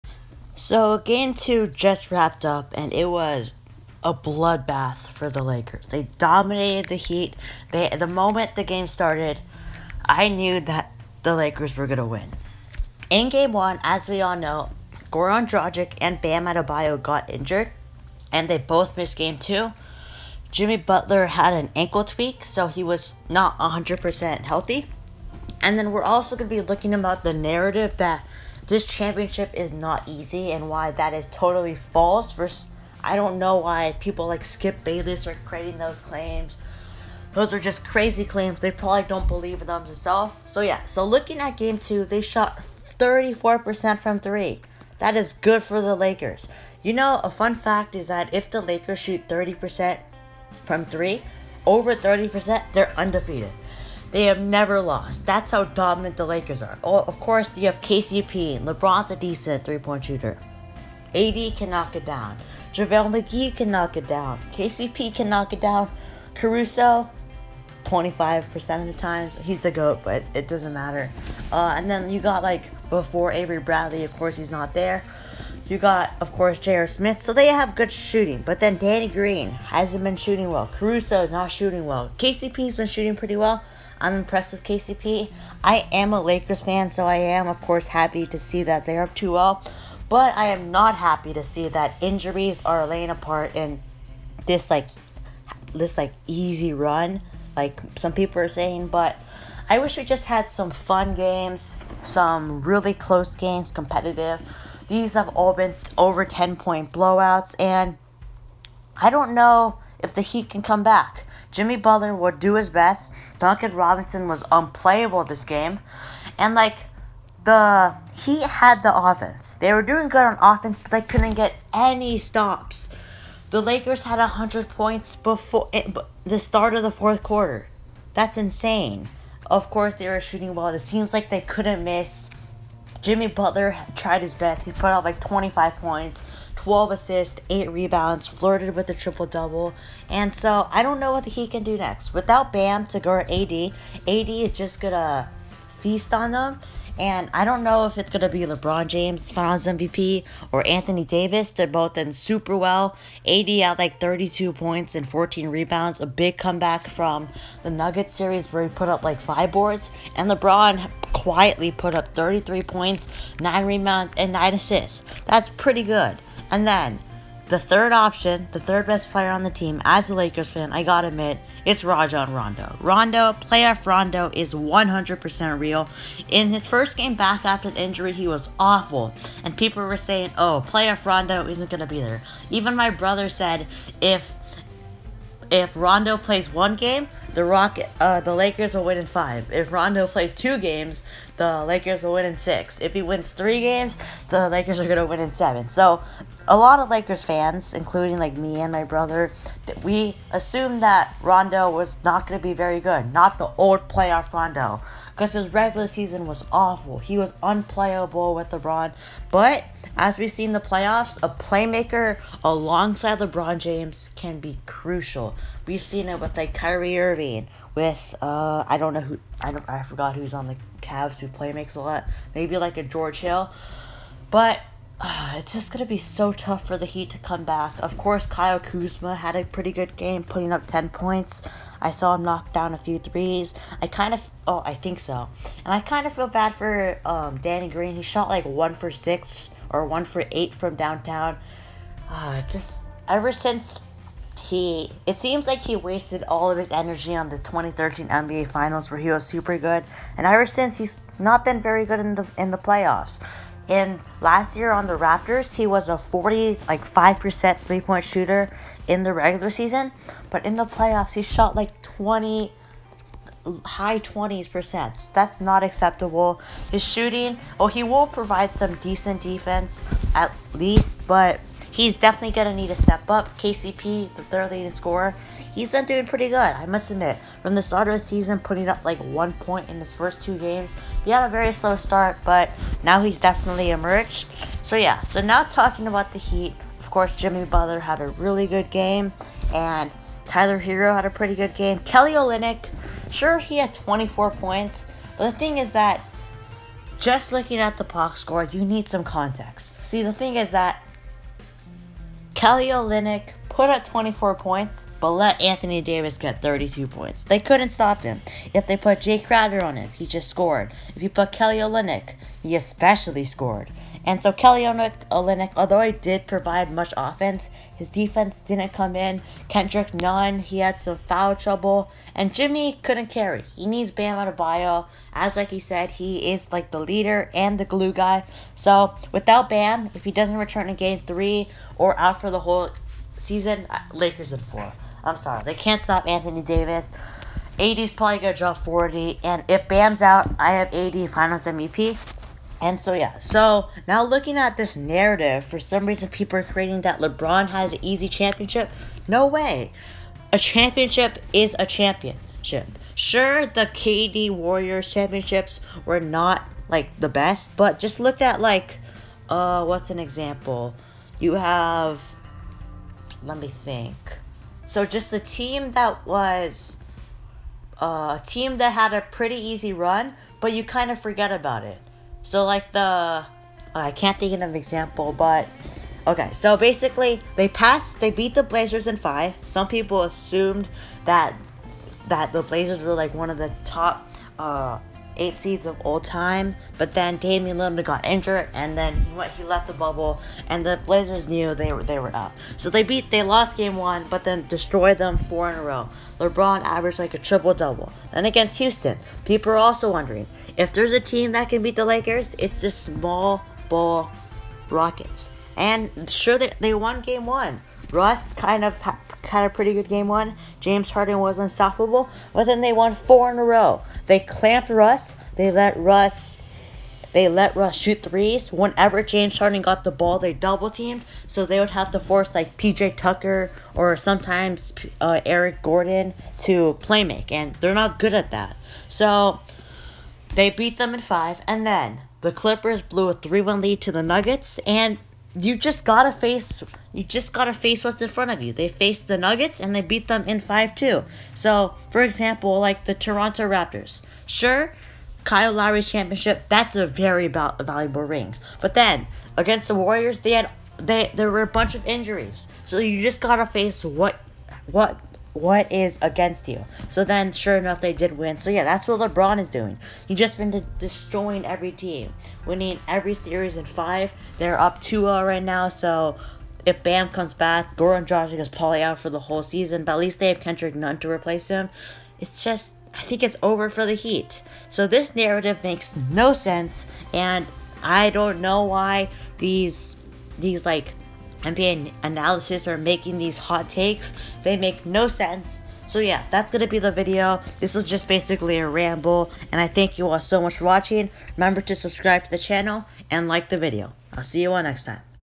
Why The Claim Lebron Had A Easy Way To The Championship Is False...
I don't get this claim
How good was Patrick Ewing